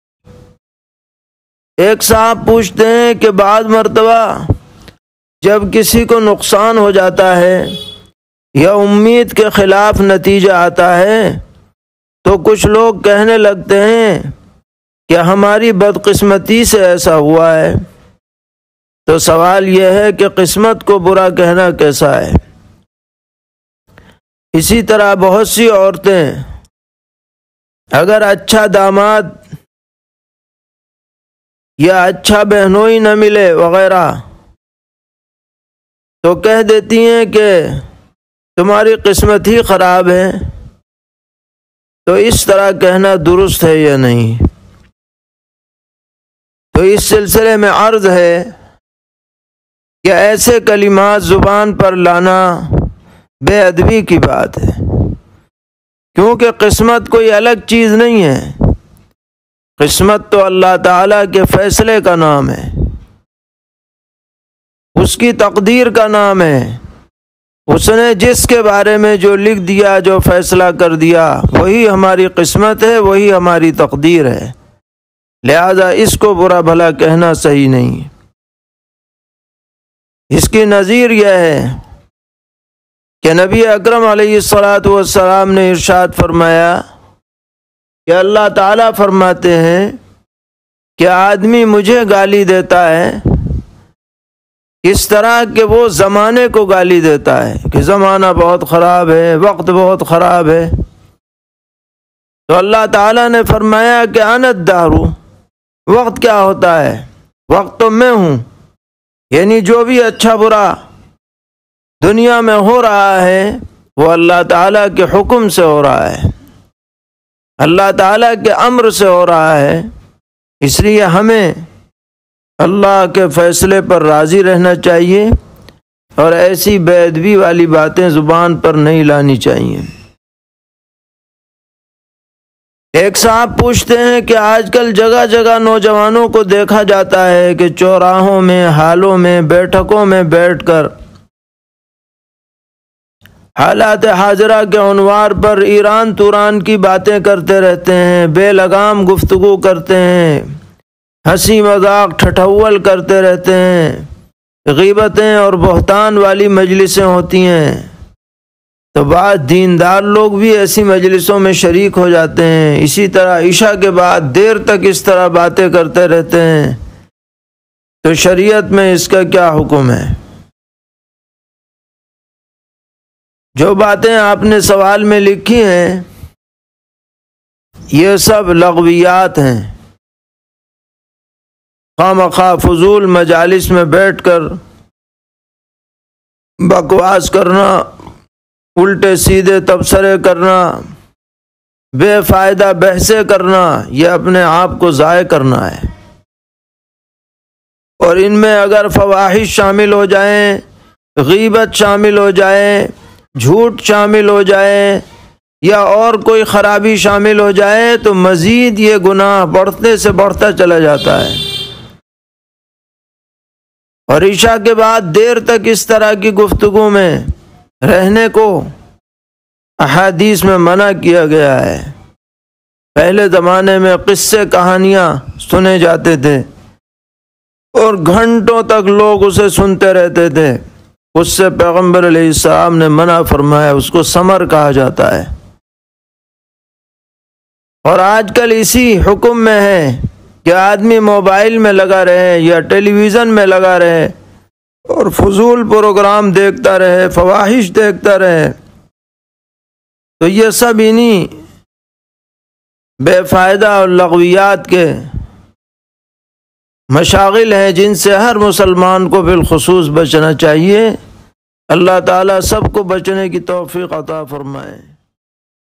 Meri Qismat Kharab Hai Kehna Durust Hai Ya Nahi, Mufti Salman Mansurpuri sahab D.B,
Hi doston assalamu alaikum
Topic of the video
Apni qismat ko bura kehna,
Qismat ko gaali dena kaisa hai,
Esha K Baad Baaten Karna Kaisa hai,
Kuch Zaroori Masail By Mufti Salman Mansurpuri sahab D.B,
Aapko a clip kaisi lagi comment karke zaroor bataayen